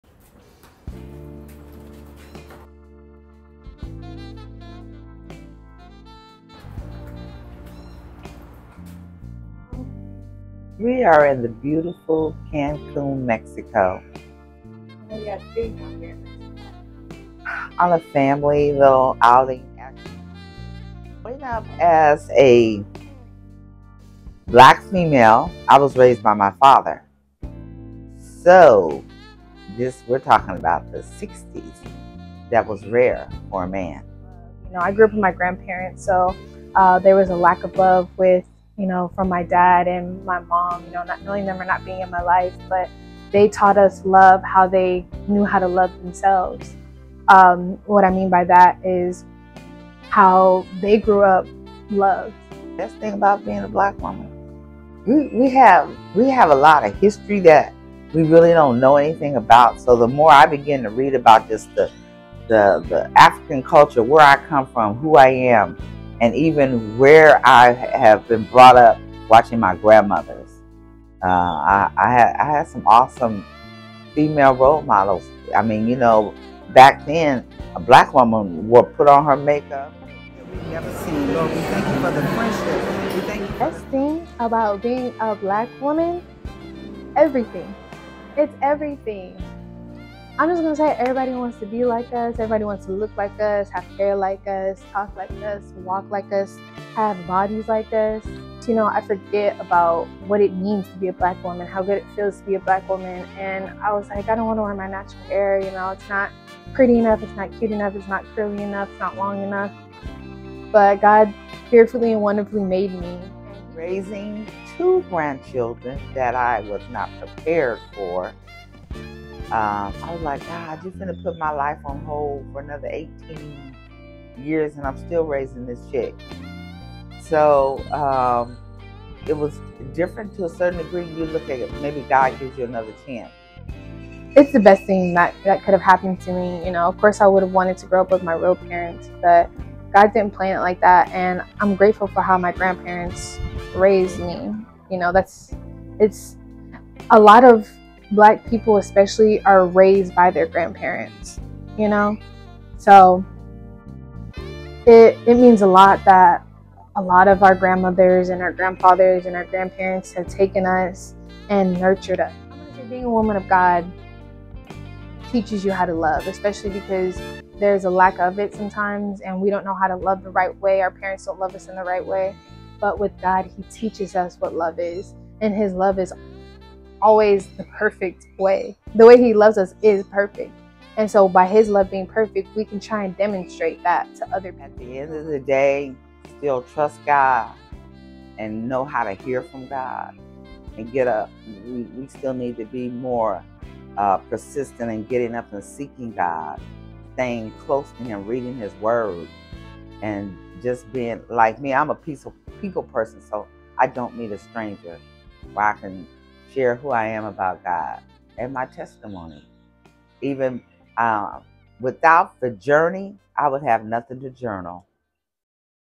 Passing the Torch: A Documentary on Black Womanhood and the Black Family
Join us on a heartfelt journey as we delve into the rich tapestry of the Black Woman and the Black Family. In this captivating documentary, spanning generations, we follow the extraordinary bond between a grandmother and granddaughter as they explore the untold stories and experiences that have shaped their lives. From the struggles and triumphs of their ancestors to the present day, witness the resilience, love, and strength that has been passed down through generations. Through intimate interviews, personal anecdotes, and a deep dive into the cultural heritage, 'Generations of Resilience' celebrates the black woman's invaluable contributions to the black family and highlights the enduring legacy that continues to inspire and uplift us all. Get ready to be inspired, enlightened, and moved by this powerful tale of heritage, unity, and the unbreakable spirit of the black community. Don't miss this captivating documentary that seeks to shine a light on the remarkable stories often overlooked but forever cherished.